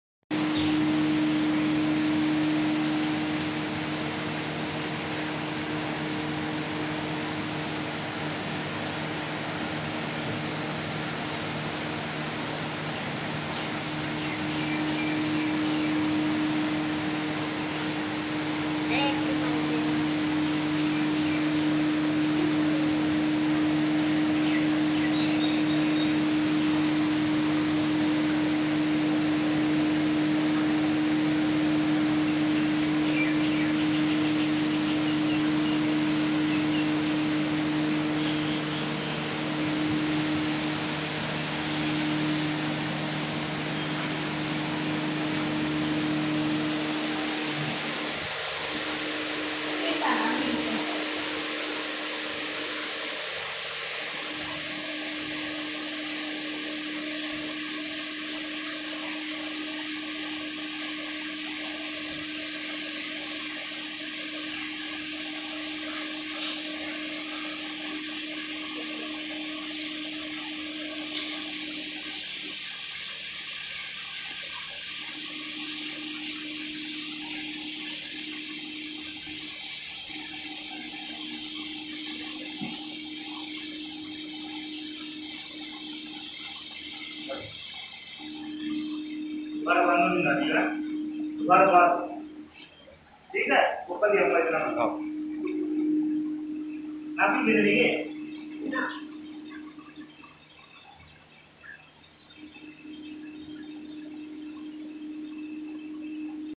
कान में छेद। Ear Piercing Without Pain। Painless Earlobes Piercing for Kids।
Kids Ear Piercing Without Pain .